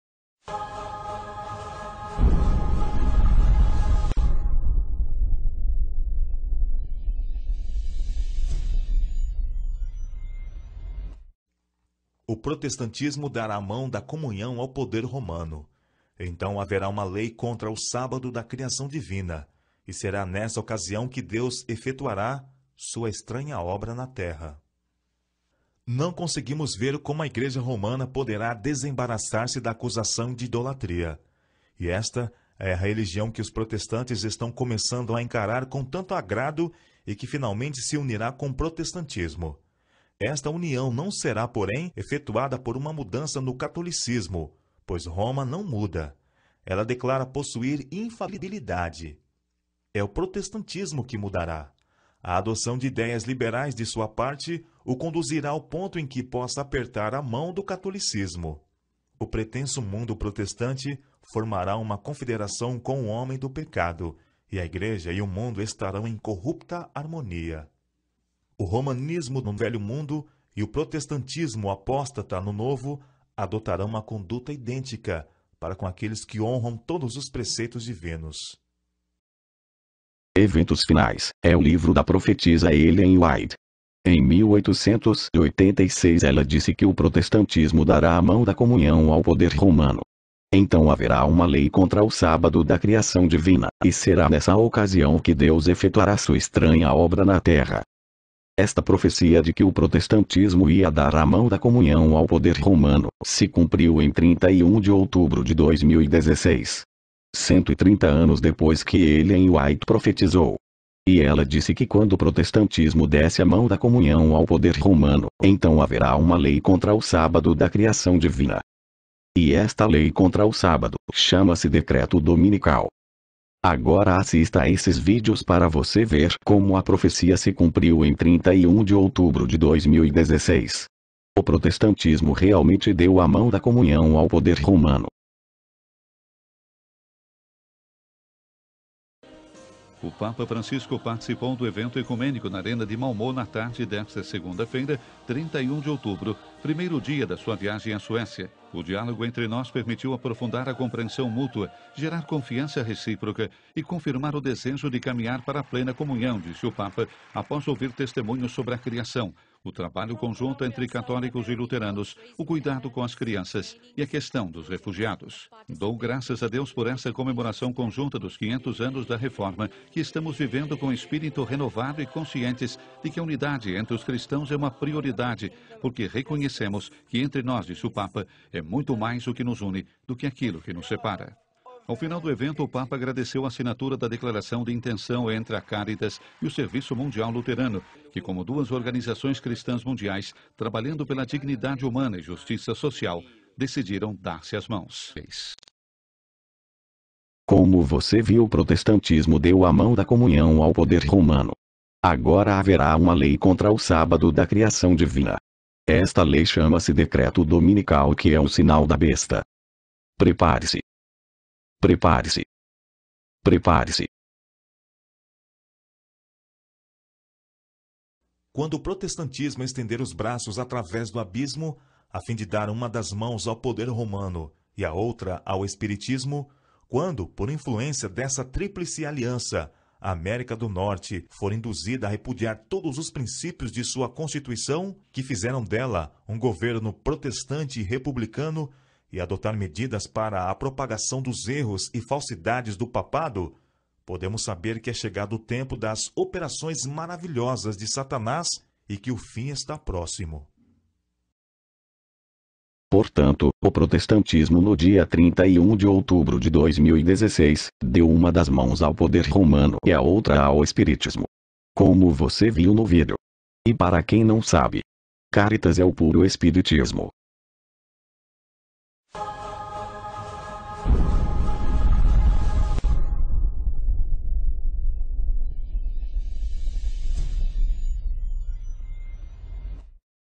ÚLTIMAS NOTÍCIAS SOBRE A NOVA ORDEM MUNDIAL 14
A pior coisa que aconteceu em 2016. agora é a marca da besta em 2017.